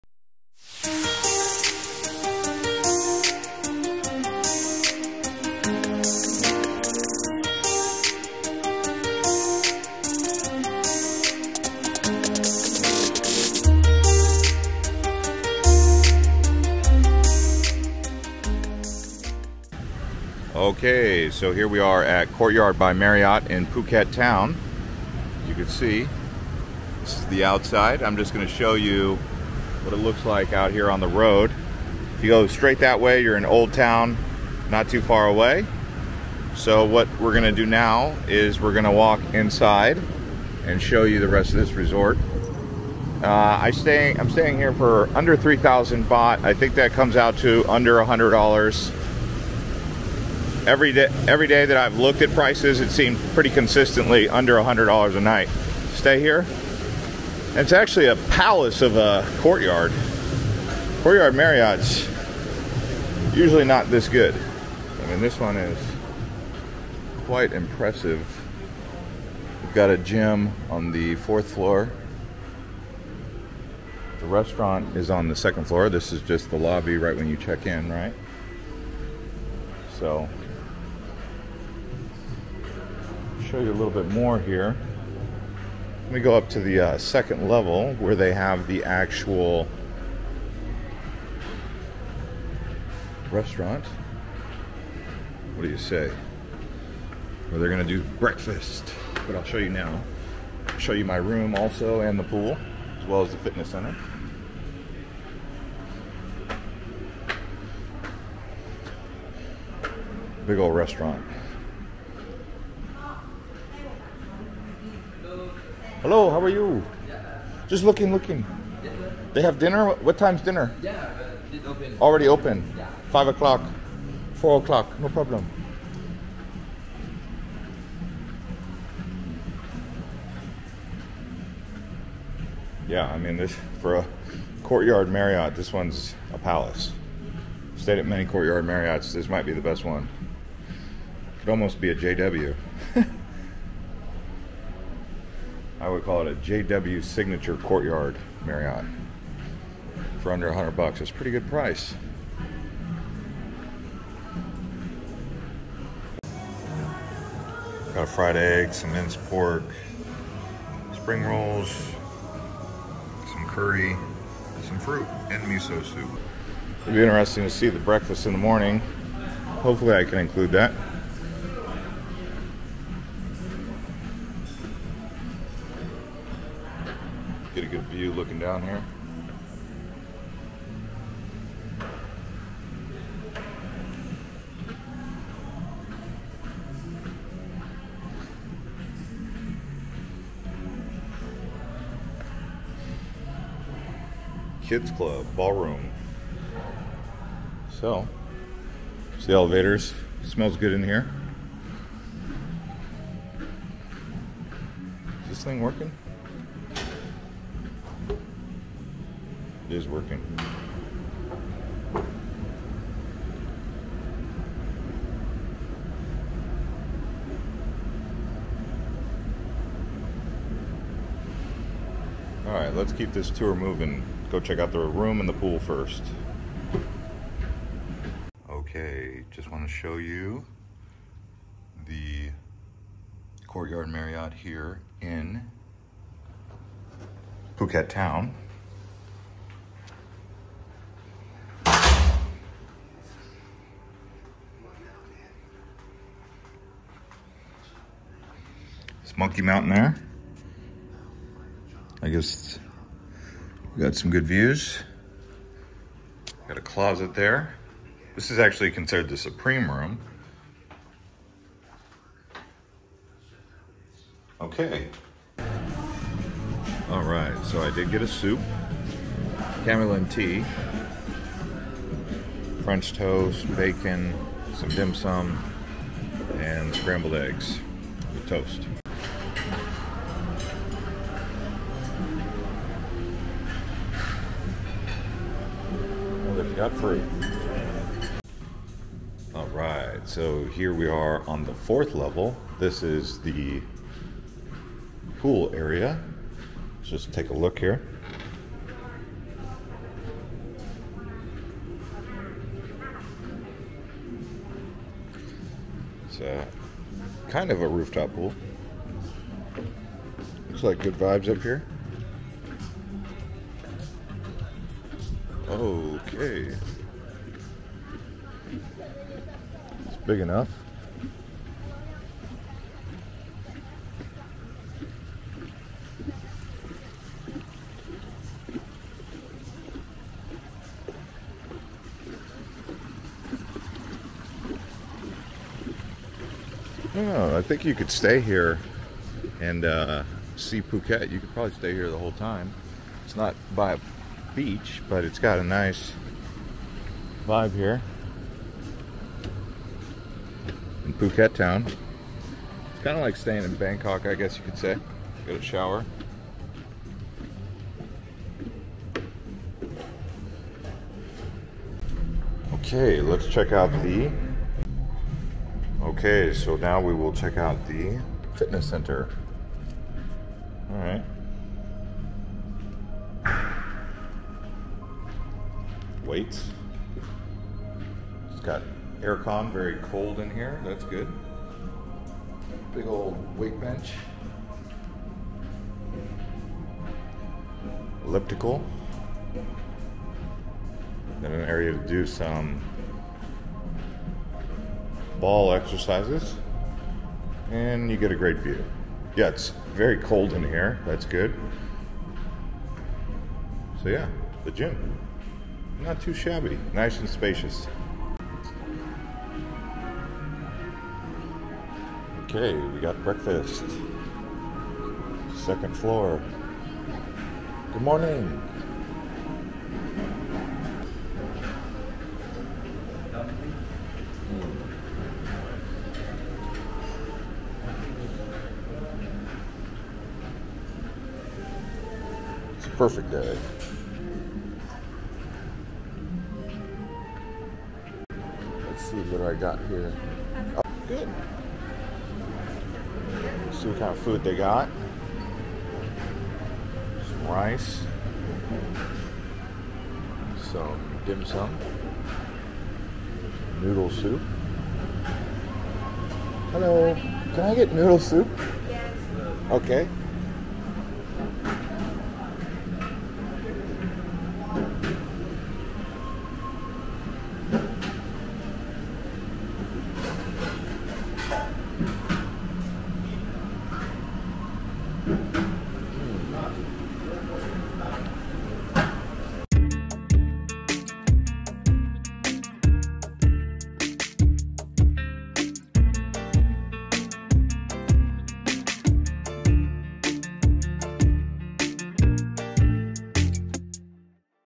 Courtyard by Marriott Phuket Town Tour 2024
In this tour of the Courtyard by Marriott Phuket Town hotel we explore this beautiful property. I stayed here for two nights and the rooms were comfortable and reasonably priced. I would stay here again. They have a pool and a gym which I do show you in the video. I also show you my room. Phuket Town is not a tourist city as much as other places on Phuket.

Island Hopper TV Hotels and Resorts is a channel that shows you the hotels and accommodation from around the world.